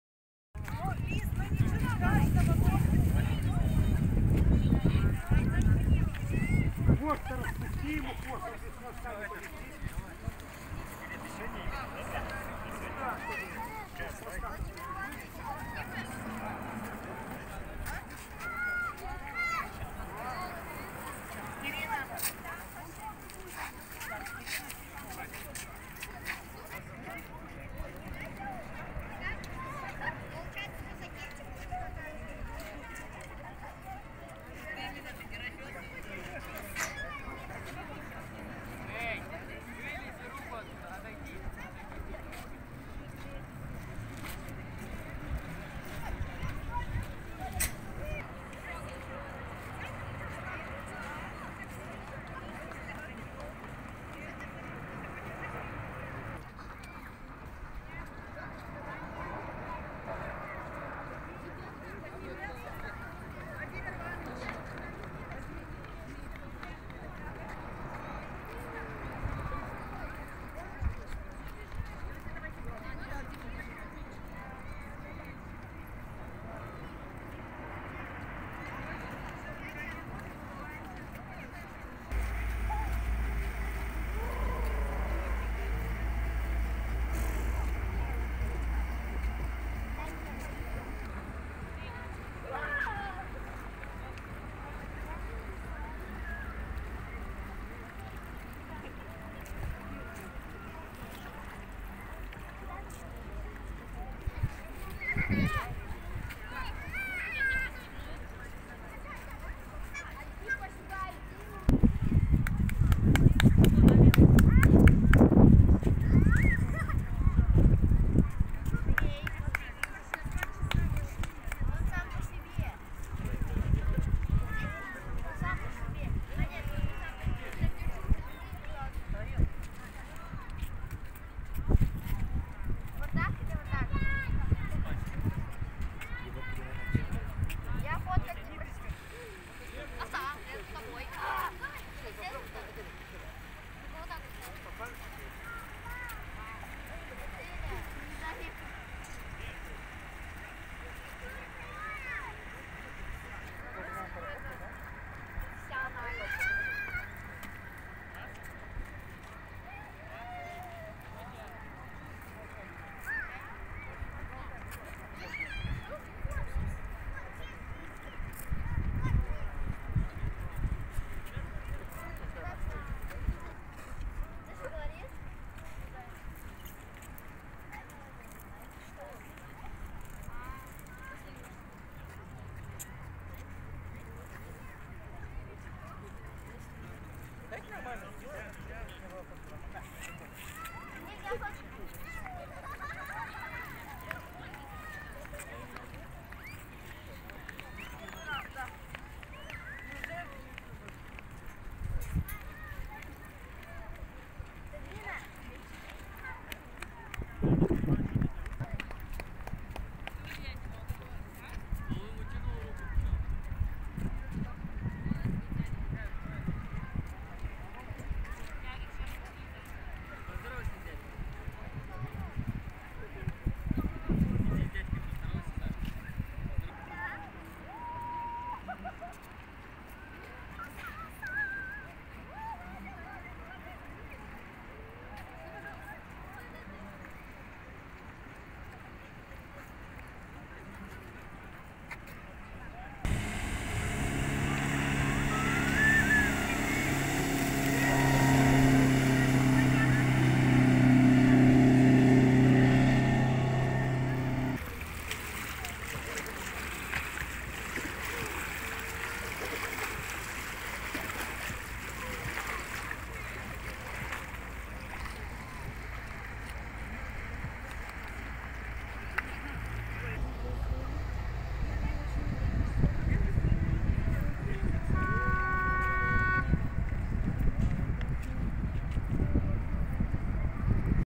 Фестиваль воздушных змеев 2022
Фестиваль воздушных змеев
в Центральном парке г.Караганда
30 июля 2022 г.

Сегодня в 12-00 в Центральном парке прошел фестиваль воздушных змеев, в котором приняли участия дворовые клубы.